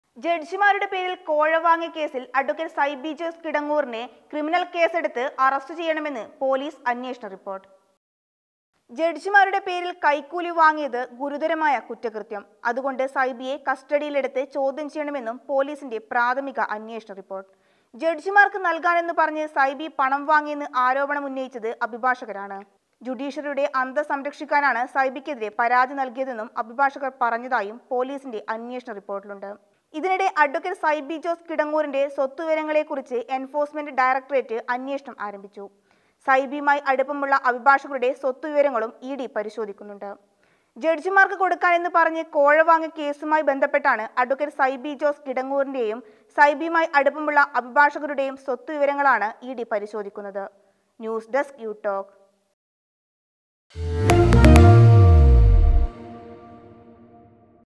സൈബി ജോസിനെതിരെ ക്രിമിനല്‍ കേസ് |Youtalk
ജഡ്ജിമാരുടെ പേരില്‍ കോഴ വാങ്ങിയ കേസില്‍ അഡ്വ സൈബി ജോസ് കിടങ്ങൂരിനെതിരെ   ക്രിമിനല്‍ കേസ് എടുത്ത്  പോലീസ് അന്വേഷണ റിപ്പോര്‍ട്ട്